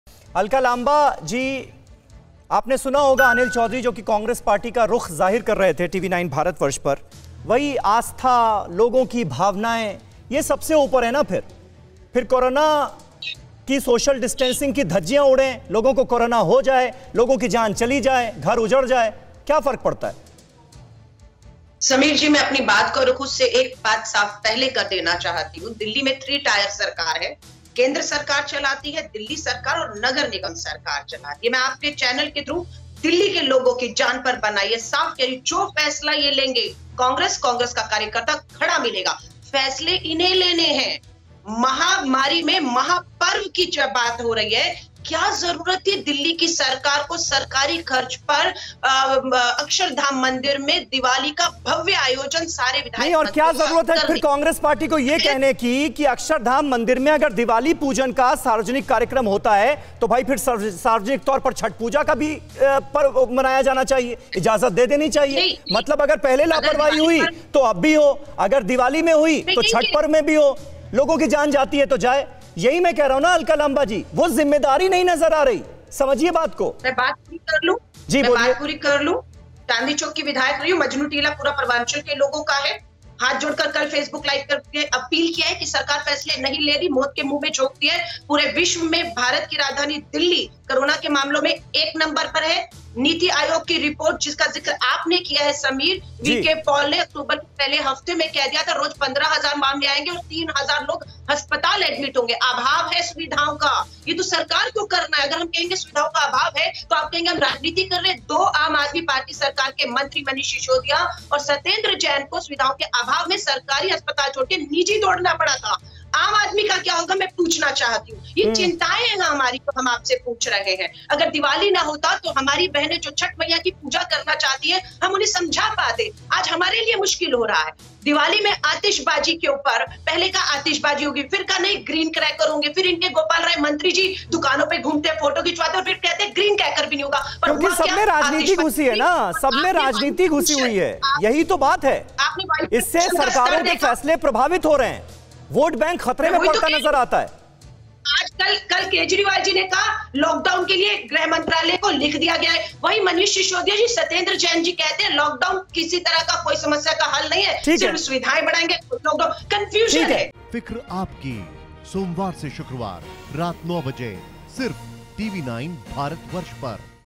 Corona काल की महामारी..भयावह हुई स्थिति
कोरोना महामारी के बीच छठ पूजा का त्योहार मनाने के लिए लोग उत्सुक हैं लेकिन केजरीवाल ने दिल्ली के हालात को देखते हुए कोताही बरतने की बात कही है.